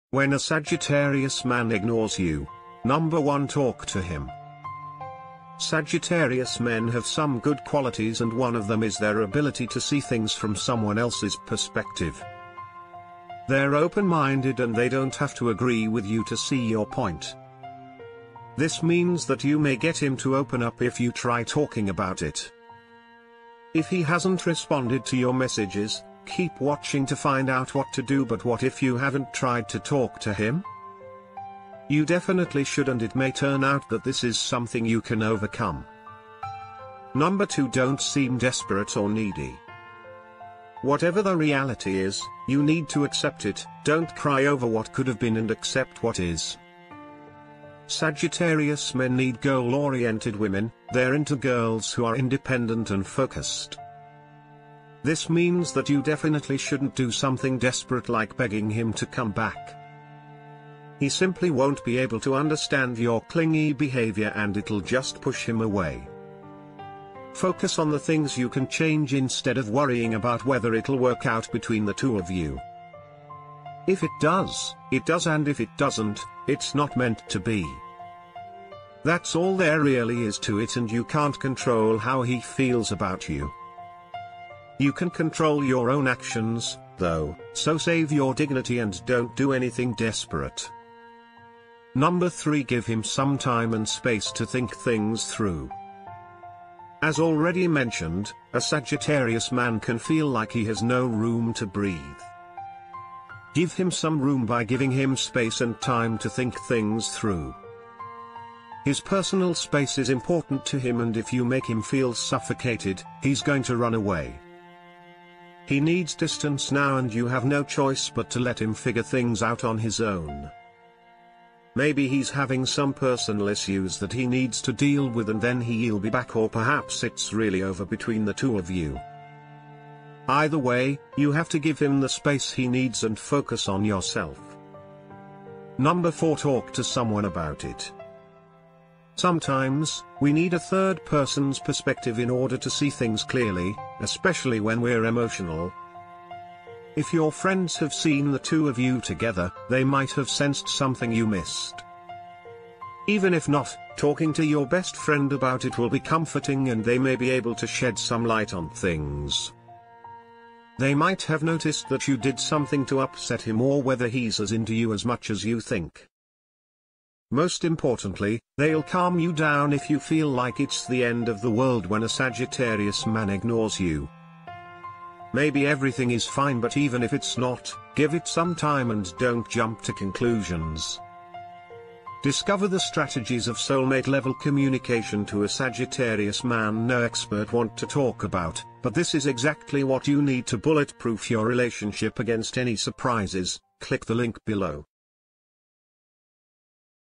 When a Sagittarius Man ignores You - What To Do?
When a Sagittarius Man ignores You - Learn how to communicate at the "soul mate level" with a Sagittarius. No expert wants to talk about it, but it's exactly what you need to protect your relationship from surprises, please follow the visit the link

# 1 Speak with him.

Sagittarius men have certain good qualities, one of which is their capacity to perceive things from another person's point of view.

They're receptive, and they don't have to agree with you to understand your argument.

This suggests that if you try to talk about it, you might be able to persuade him to open up.

If he hasn't responded to your messages, keep an eye on him to see what you should do, but what if you haven't tried to communicate to him?

You should absolutely do so, and it may find out that this is something you can overcome.

# 2 Don't appear desperate or in need.

Whatever the reality is, you must accept it. You must not mourn over what could have been and instead accept what is.

Sagittarius men want goal-oriented women. They prefer ladies that are self-sufficient and motivated.

This means you should avoid desperate measures such as pleading with him to return.

He simply won't understand your clingy behavior, and it will push him away.

Instead of stressing about whether or not it will work out between the two of you, concentrate on the things you can alter.

If it does, it is intended to be; if it does not, it is not meant to be.

That's all there is to it, and you have no control over how he feels about you.

You can, however, control your own behavior, so keep your dignity and avoid doing something desperate.

#3 Give him some space and time to consider things through.

As previously stated, a Sagittarius guy may feel as if he has no room to breathe.

Allow him some breathing room by providing him with space and time to consider things through.

His personal space is vital to him, and if he feels suffocated, he will flee.

He requires space at this point, and you have no choice but to let him think things out on his own.

Perhaps he has some personal difficulties that he has to address before returning, or perhaps it's completely over between the two of you.

In any case, you must give him the space he requires while focusing on yourself.

#4 Discuss it with someone.

Sometimes, especially when we're emotional, we need a third person's perspective to see things properly.

If your buddies saw the two of you together, they might have picked up on something you didn't.

Even if you don't, talking about it with your best friend will be reassuring, and they may be able to cast some light on the situation.

They may have seen that you did anything to irritate him or that he isn't as into you as you believe.

Most significantly, they'll calm you down if a Sagittarius man ignores you and you feel like it's the end of the world.

Everything may be alright, but if it isn't, give it some time and don't jump to conclusions.